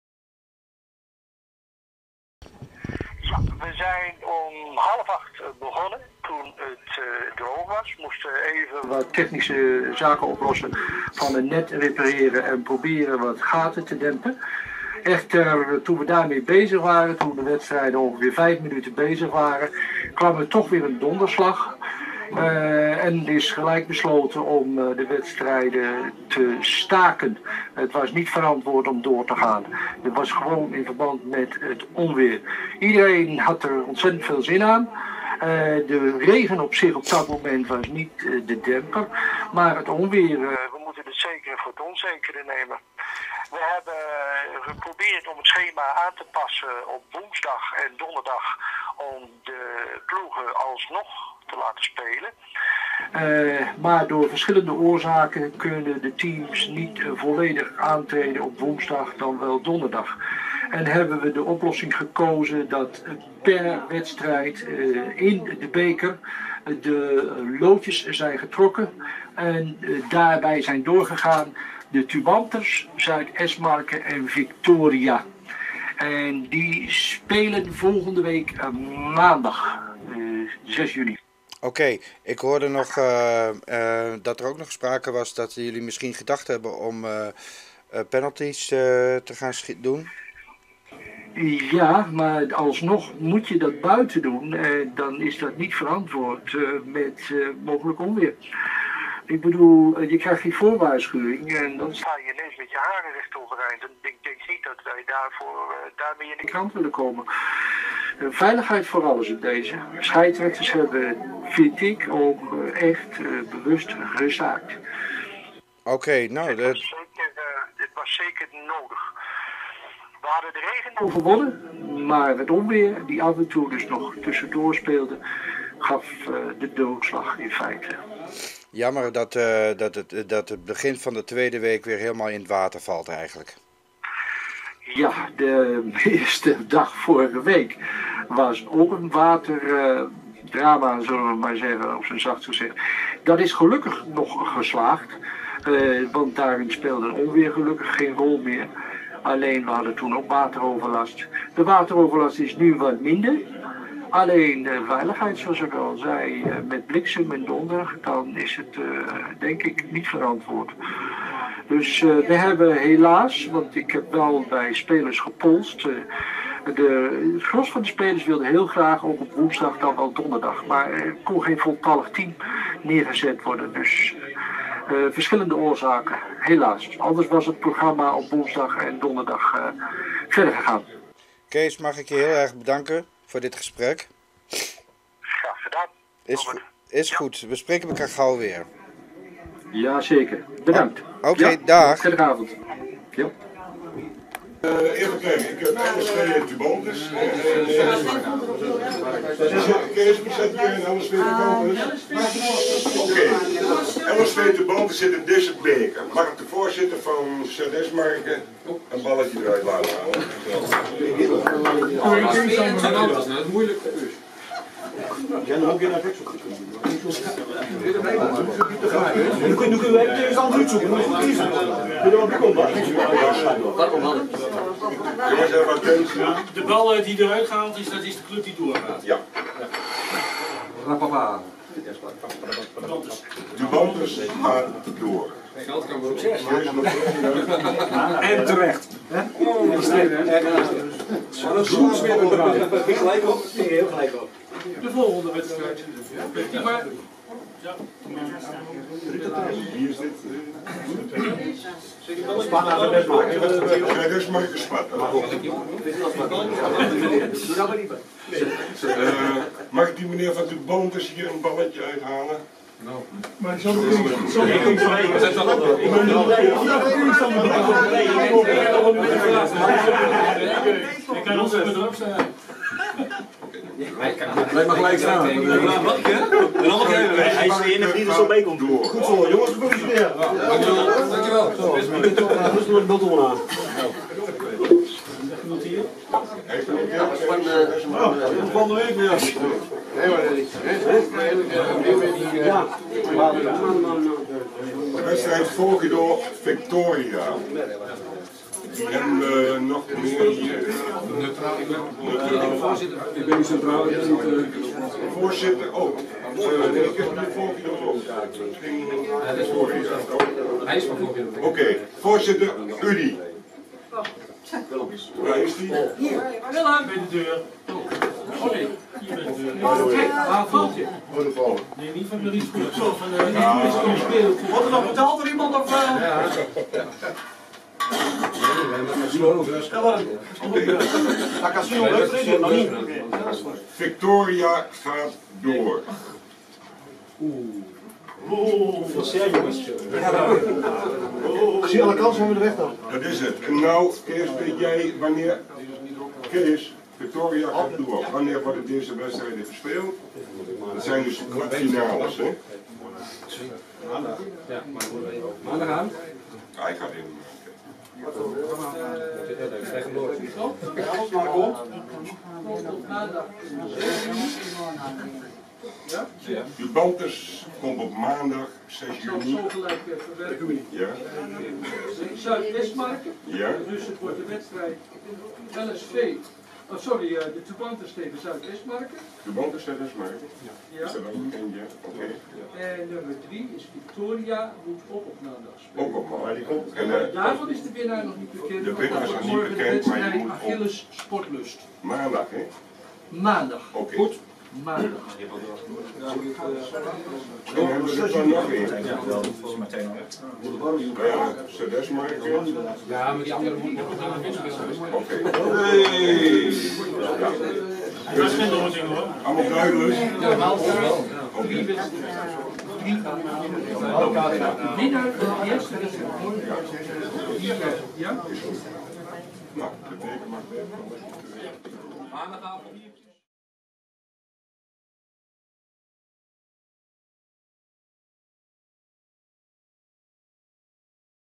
Marinus Goedhart toernooi Maandag 30 mei wedstrijden afgelast
Marinus Goedhart toernooi Maandag 30 mei 2016 wedstrijden afgelast vanwege onweersbuien.